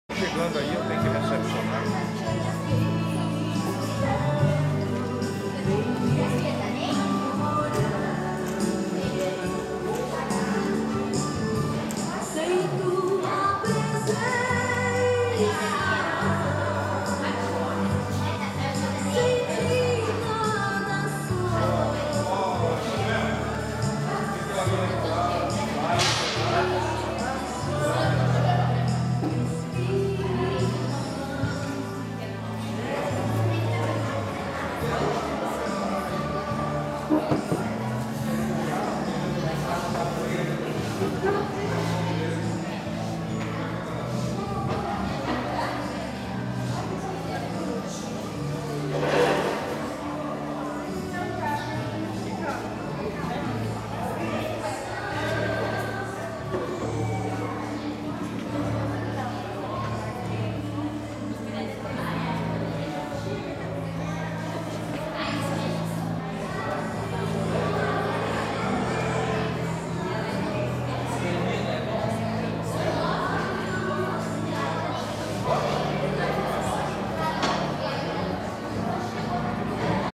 M2U01575 Formiguinhas Vila Zumbi - escola profissionalizante
Natal das Crianças de 2008 em Colombo, na Vila Zumbi, festa idealizada pelo Bispo José Dutra, criador das Formiguinhas da Vila Zumbi, entidade formada com o apoio da FAE, LIONS e outras.